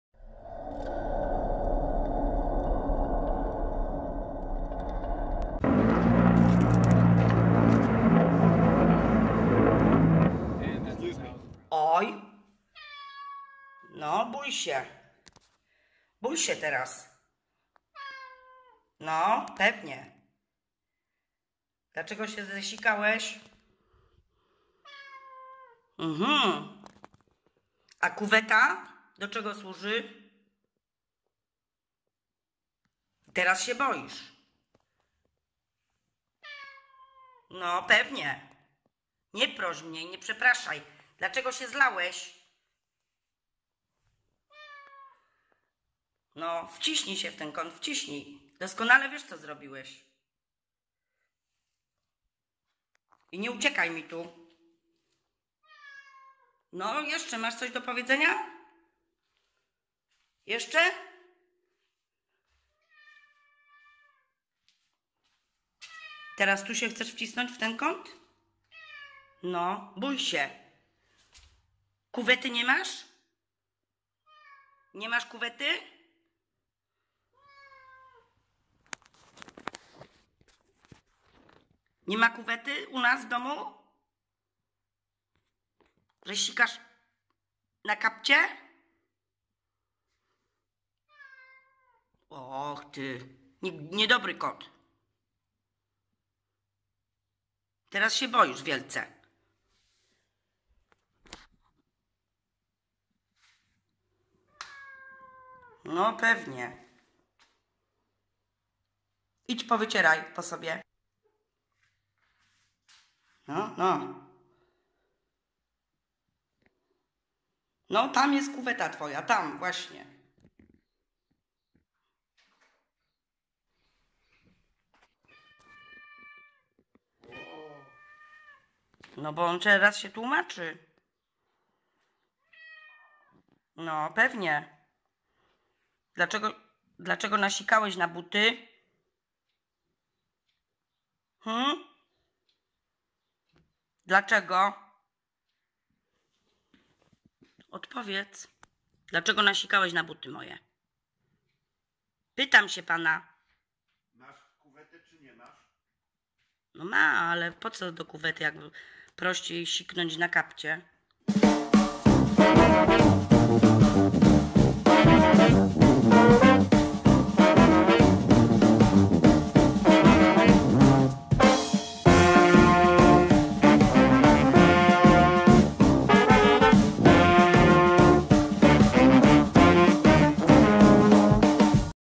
Rozmowy z kotem 2, czyli Miki w poważnych opałach
Elokwencja mojego kotka Mikiego ratuje go z poważnych opresji...